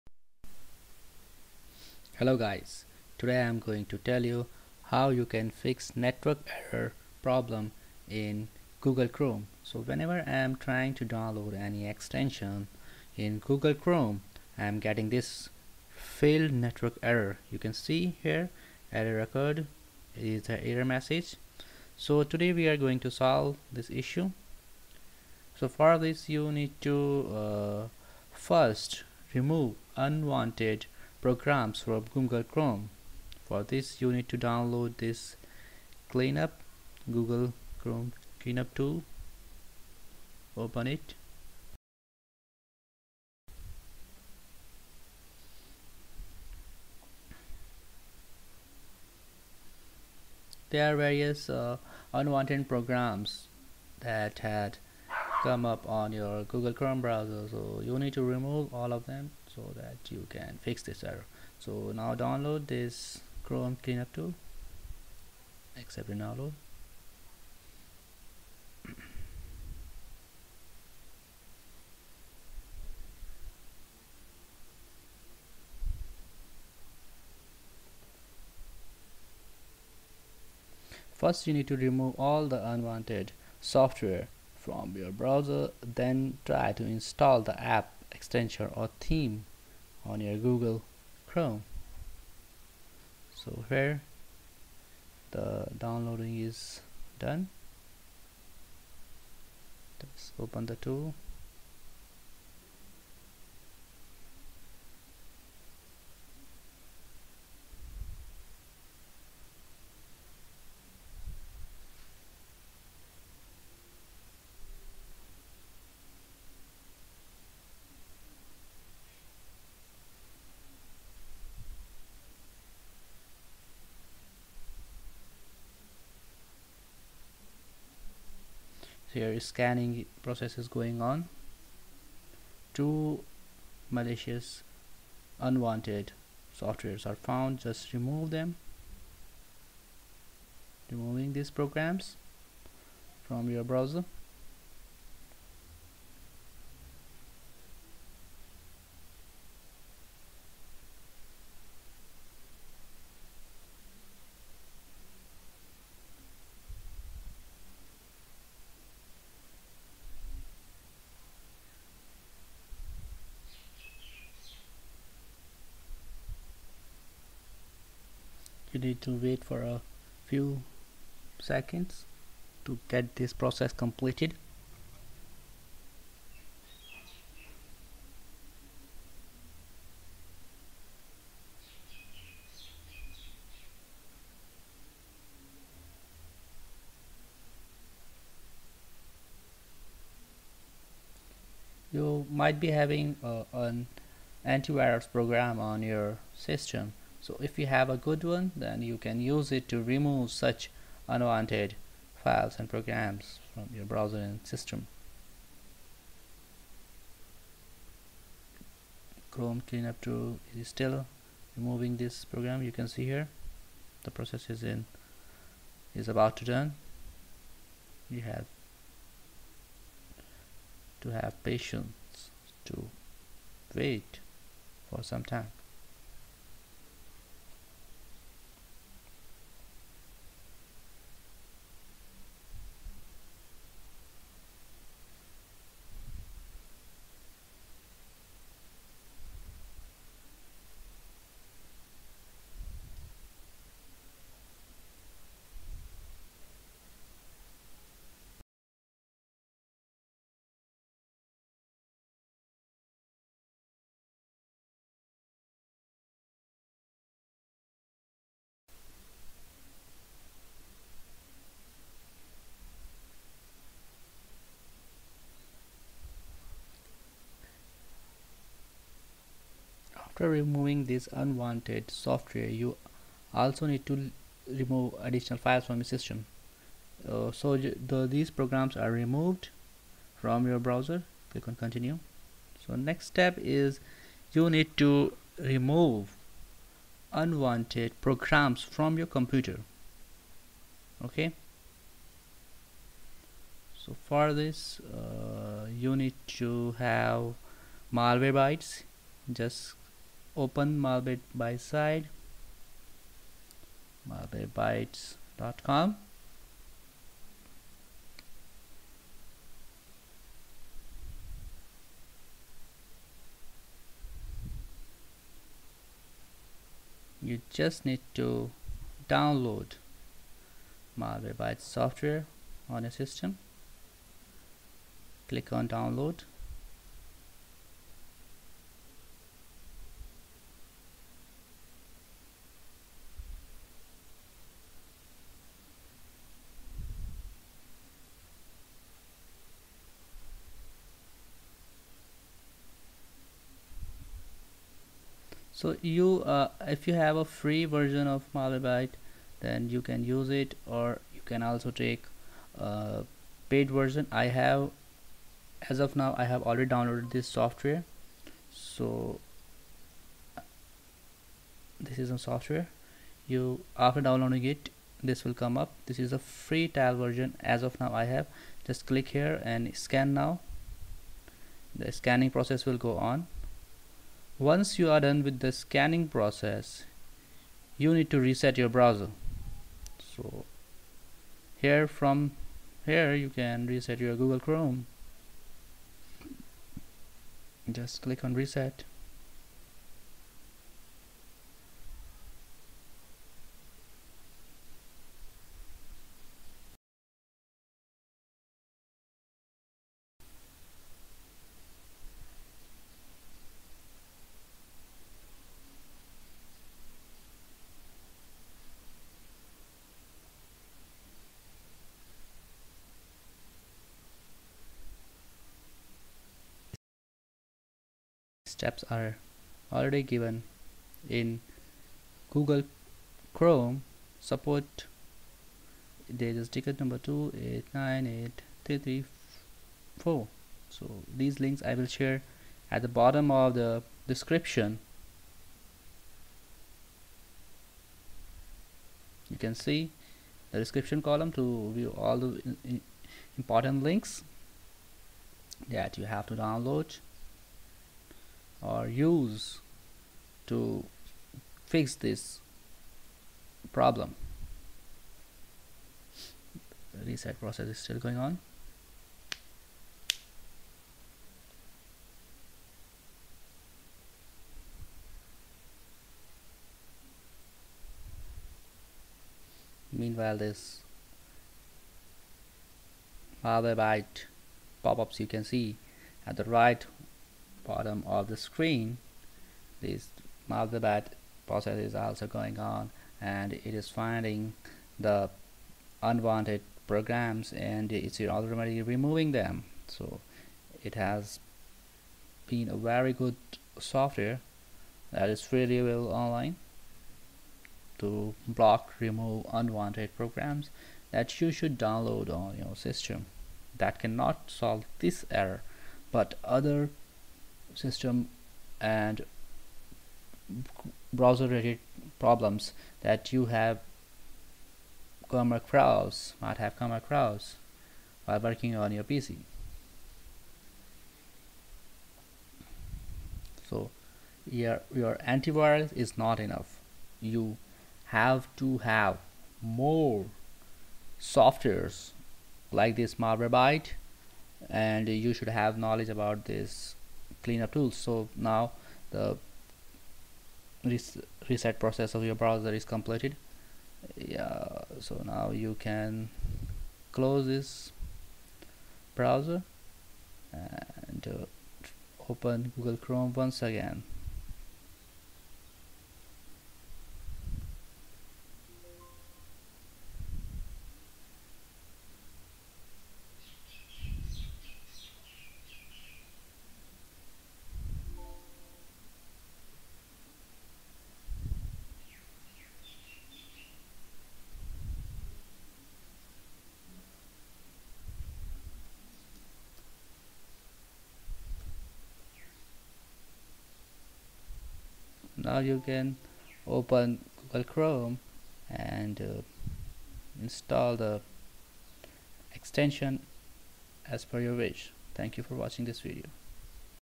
Failed - Network Error in Google Chrome [Solution] - Without Uninstalling Google Chrome
Steps to fix Failed - Network Error in Google Chrome. The error occurs while installing app or extension in Google Chrome.